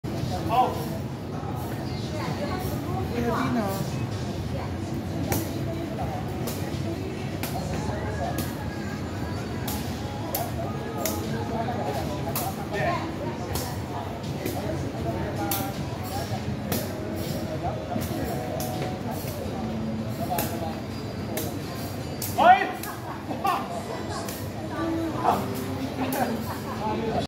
1468 -20231214F - 榮哥接受香港01訪問@歌賦善慶街踢毽子Hong Kong Gough Shin Hing Street play shuttlecock jianzi sports
Activity date : 14 Dec 2023
Upload date : 15 Dec 2023